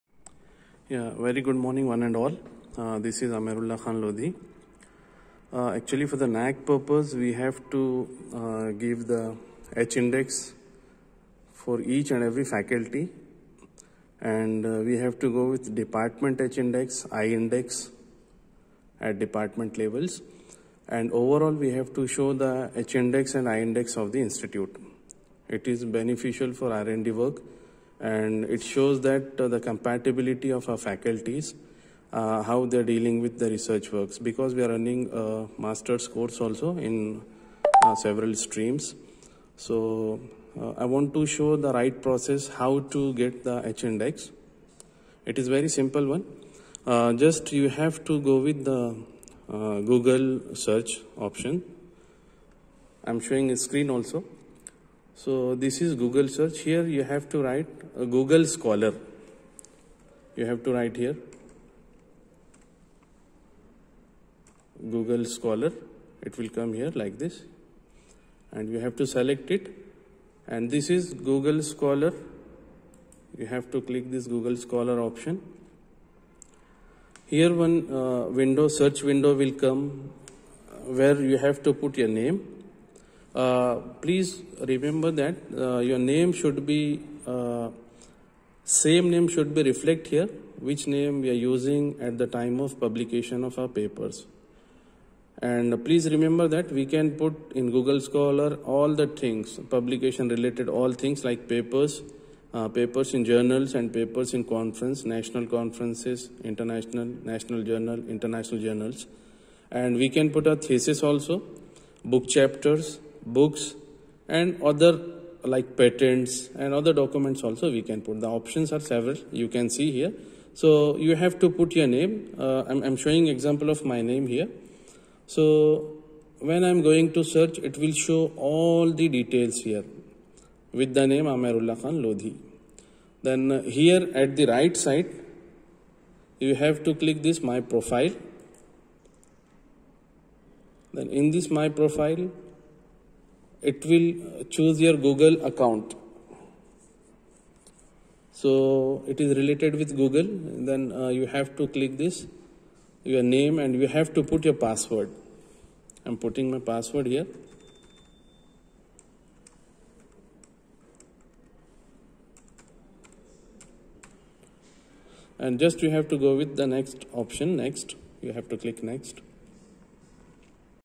How to Create Google Scholar Account Google Scholar Account Creation H-index I-10 index
This video will give the basic information about creating a Google Scholar account. A scholar can easily make his or her account. This is very useful for finding the H-Index and I10-Index of the individuals.